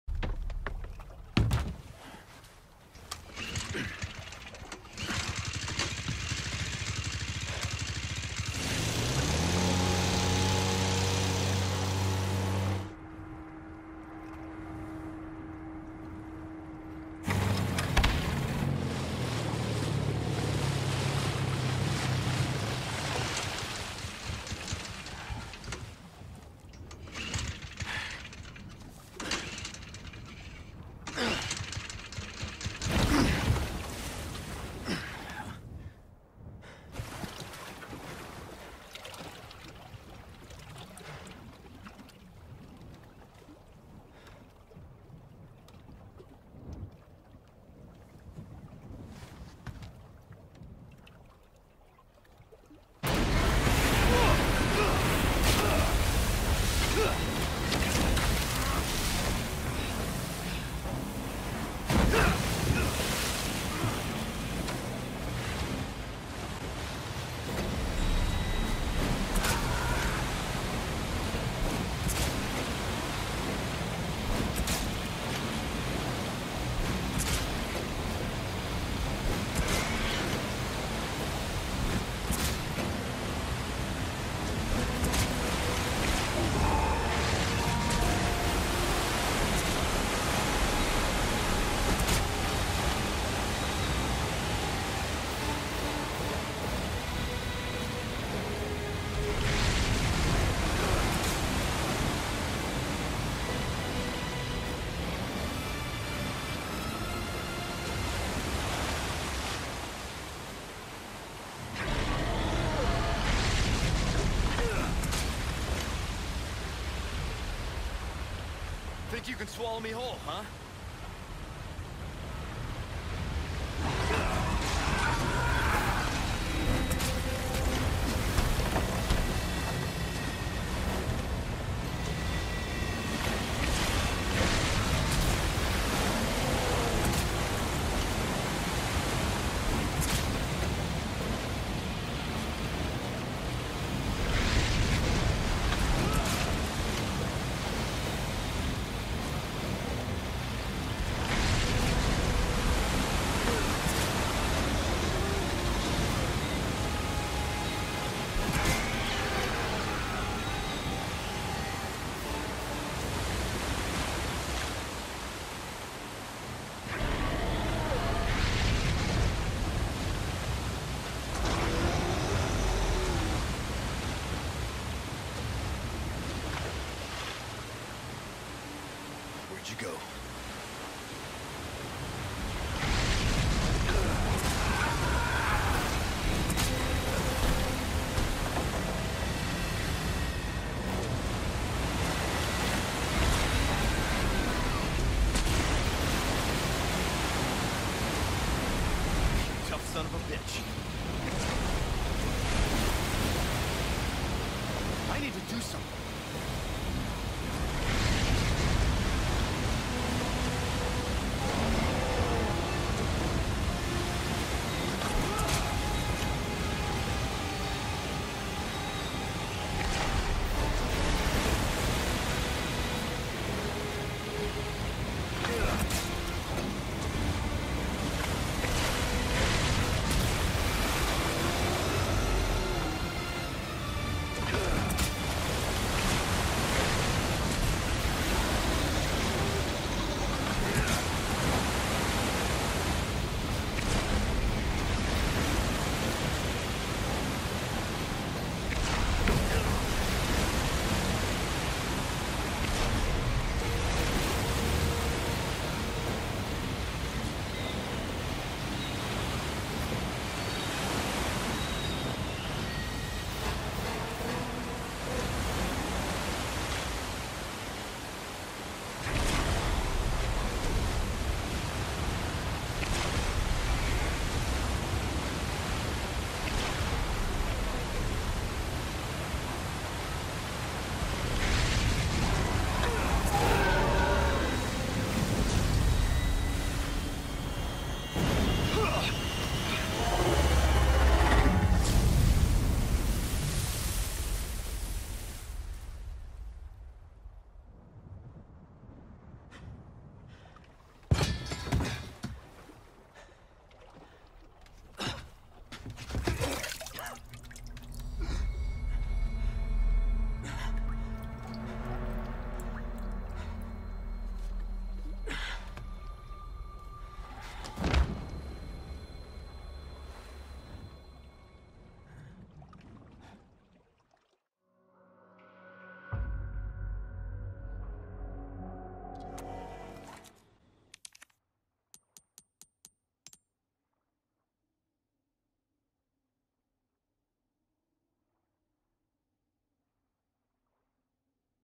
In Tune with the Harpoon
Earning challenge points for beating Del Lago without missing a throw!